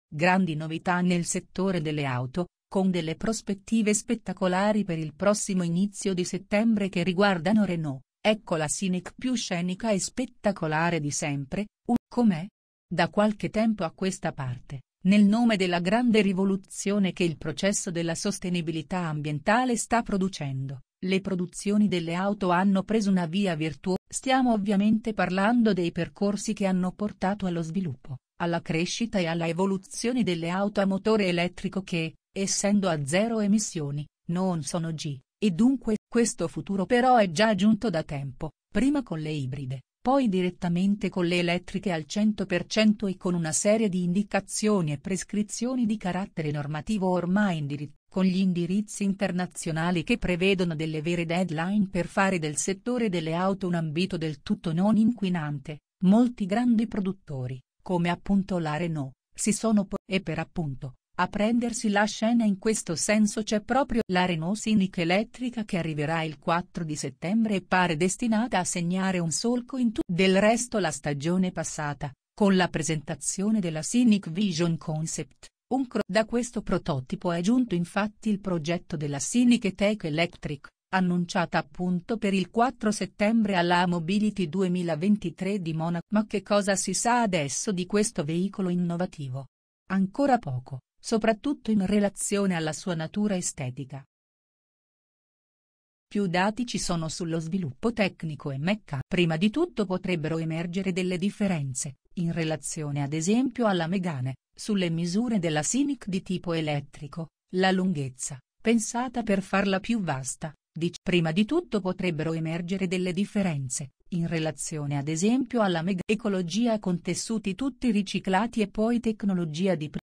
Renault Scenic e R5, a settembre arriva il Suv medio francese I Sembra una Bmw ma costa la metà
Renault Scenic e R5, a settembre arriva il Suv medio francese I Sembra una Bmw ma costa la metà Grandi novità nel settore delle auto, con delle prospettive spettacolari per il prossimo inizio di settembre che riguardano Renault: ecco la Scenic più ‘scenica’ e spettacolare di sempre: uno schianto. Com’è? Da qualche tempo a questa parte, nel nome della grande rivoluzione che il processo della sostenibilità ambientale sta producendo, le produzioni delle auto hanno preso una via virtuosa: e Renault non fa eccezioni. Stiamo ovviamente parlando dei percorsi che hanno portato allo sviluppo, alla crescita e alla evoluzione delle auto a motore elettrico che, essendo a zero emissioni, non sono g, e dunque sono il ‘futuro’. Questo futuro però è già giunto da tempo: prima con le ibride, poi direttamente con le elettriche al cento per cento e con una serie di indicazioni e prescrizioni di carattere normativo ormai in dirittura di arrivo. Con gli indirizzi internazionali che prevedono delle vere